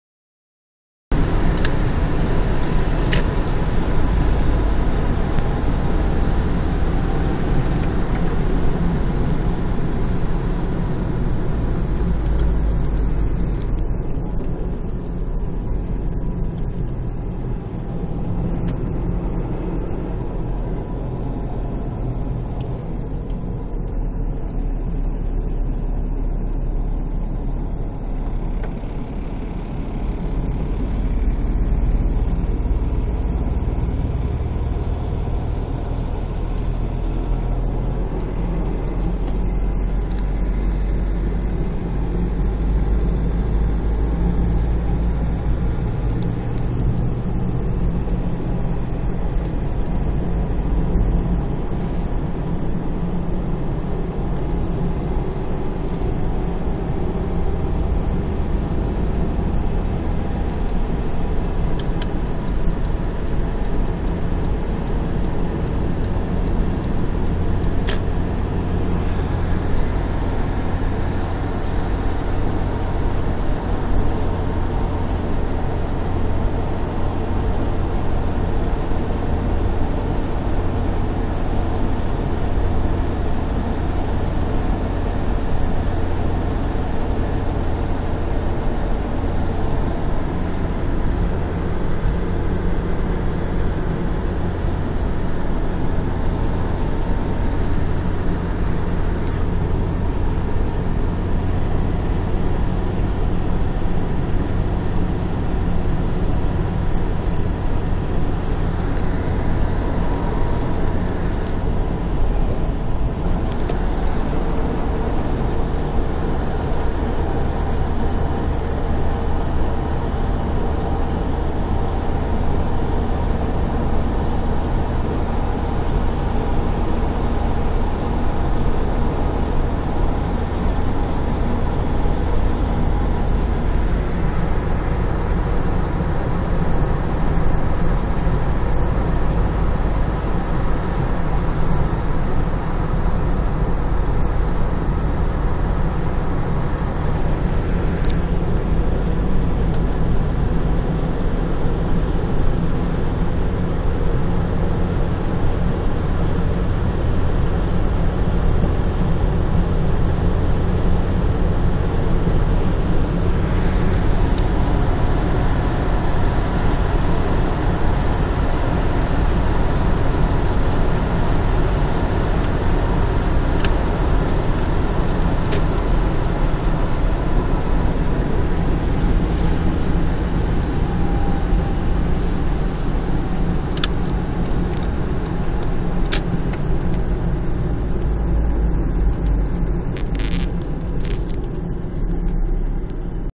Łącznik Długołęka
Społeczny monitoring AOW (Łącznik Długołęka) pod kątem realizacji umowy na utrzymanie trasy